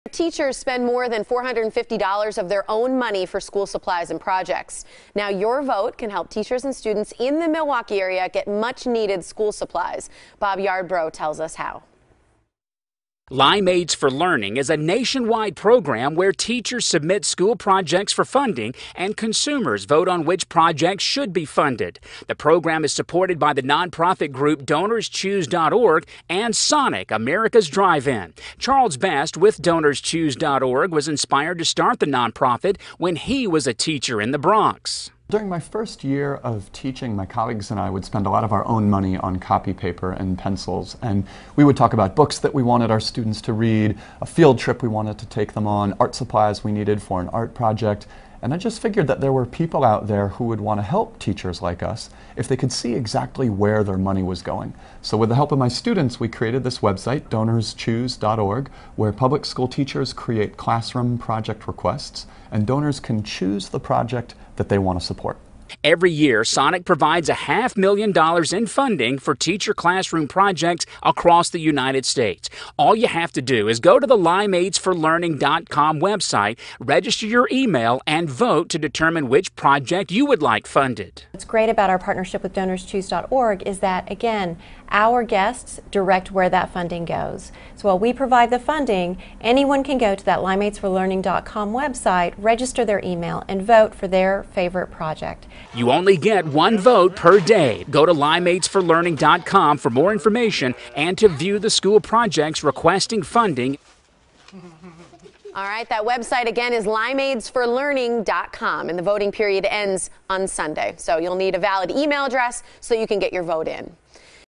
How You Can Help in the Classroom
Every year, it's estimated each teacher spends $458 of their own money on supplies for their classrooms. As a history teacher in the Bronx, Charles Best was doing just that, which inspired him to create DonorsChoose.org.  This website allows teachers to post classroom project requests that anyone can help fund donations big and small. Since 2010, DonorsChoose.org has raised $259,862,253, funded 487,571 projects, and helped 12,229,751 public school students.